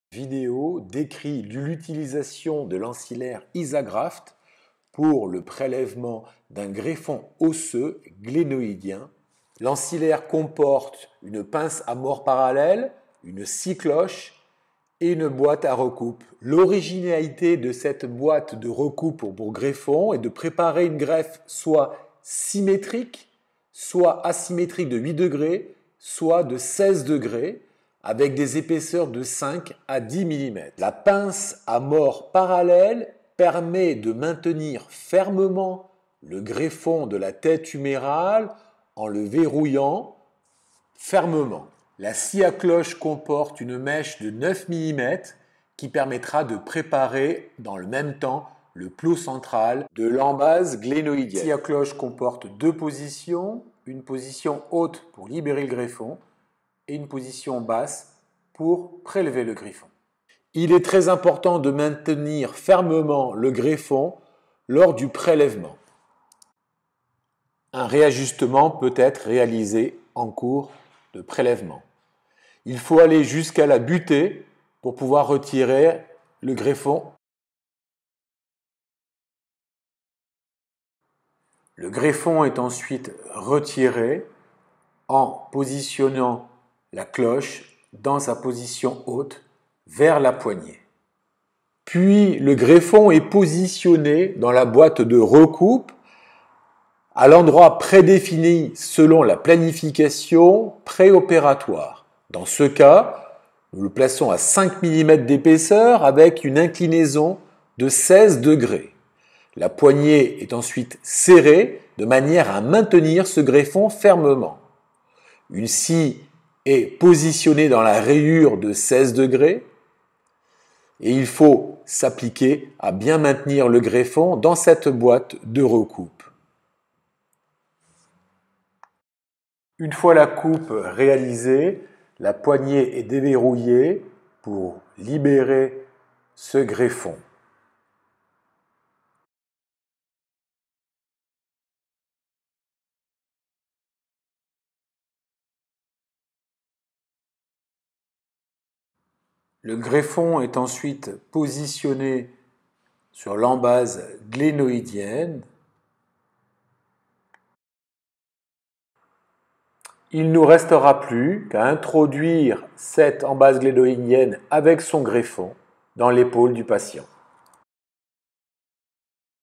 Préparation du greffon glénoïdien avec l'ancillaire ISAgraft by Shoulder Friends Institute.
Christophe Charousset nous explique la technique de préparation du greffon glénoïdien facilitée par l'ancillaire ISAgraft dédié à la technique et développé par Shoulder Friends Institute